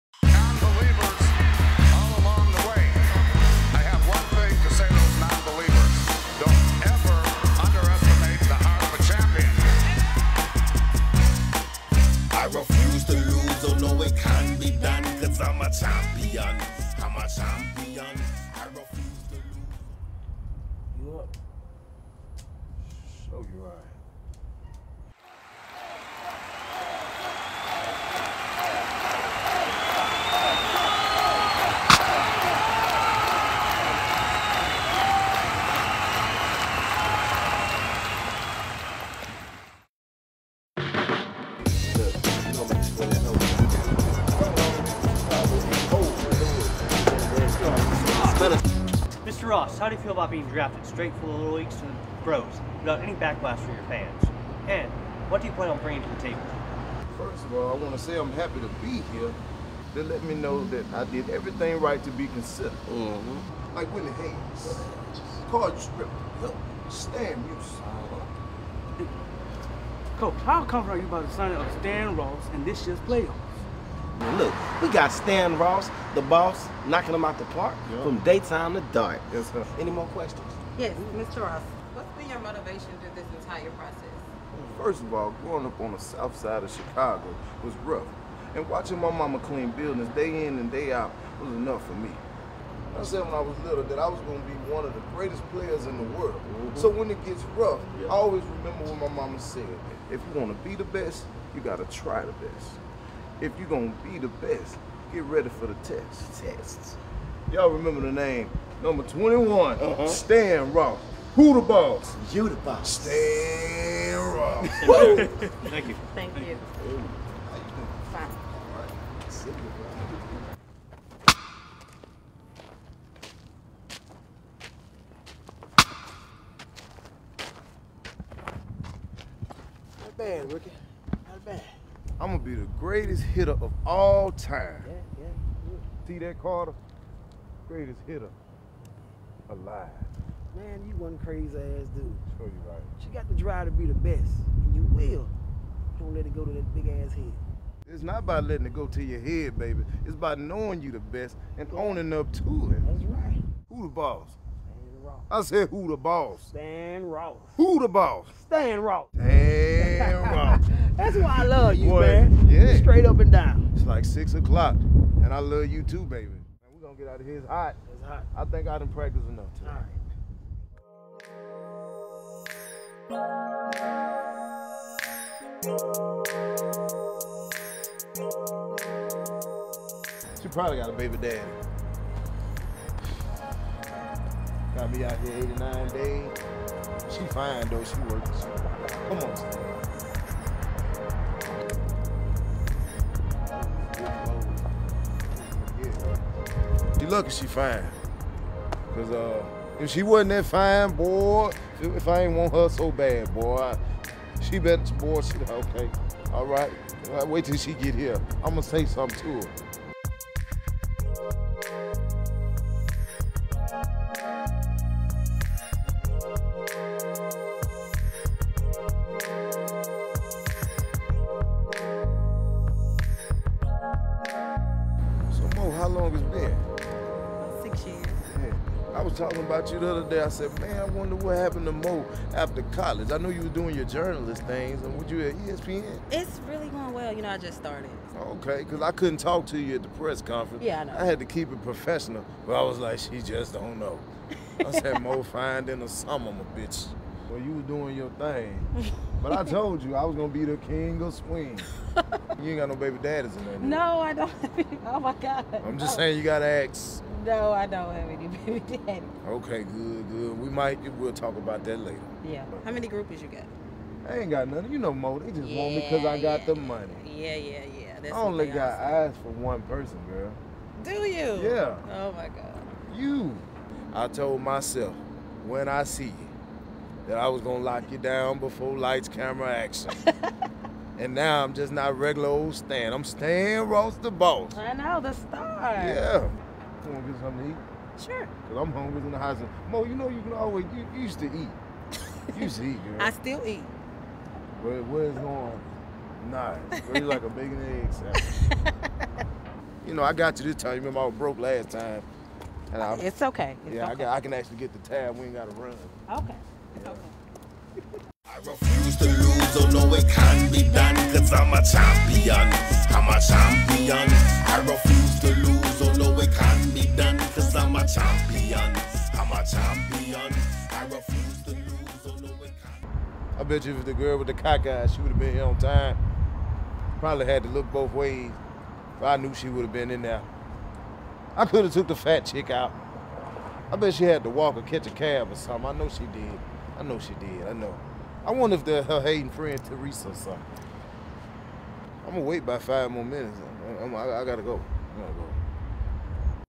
comedian Jeremy Unmask(Mr.3000 the Rookie year mini skit)
Comedian Jeremy Unmask stars in one of his favorite Bernie Mac remake films in a mini movie skit. Take a trip back to when Mr.3000 was just Stan Ross and Maureen was just starting her career as well at ESPN.This just a little tell of what Mr.3000 was like on his journey to the top..PutMeOnSteve.